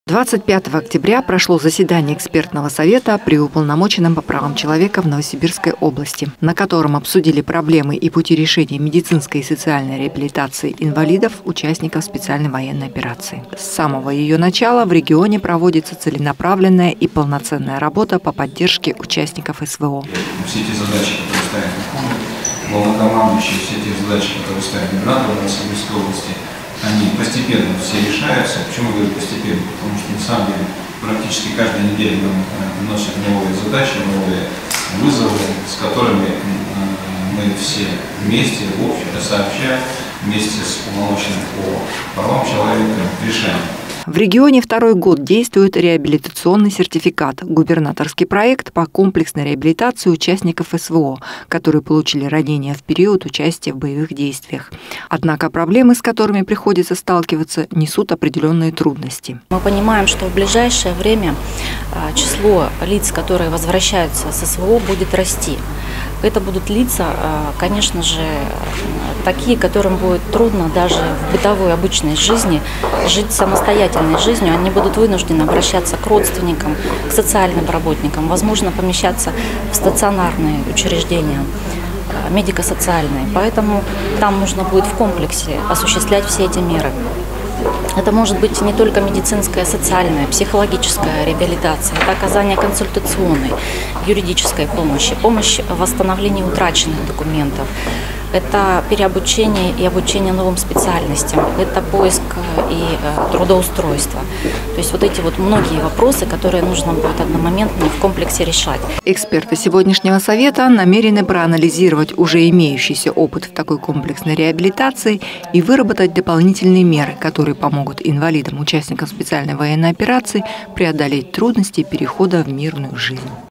Реабилитацию инвалидов-участников СВО обсудили в Новосибирске // "Новости 49" 25.10.24
Демобилизованные участники специальной военной операции, которым повезло выжить и вернуться домой, часто становятся инвалидами. Реабилитация таких бойцов сегодня выходит на первый план. В регионе работа в этом направлении ведется с начала спецоперации. Усовершенствовать и выработать дополнительные меры в этой работе – задача, которая сегодня стоит перед государственными органами. Как этого добиться, обсудили 25 октября на заседании экспертного совета при уполномоченном по правам человека в Новосибирской области.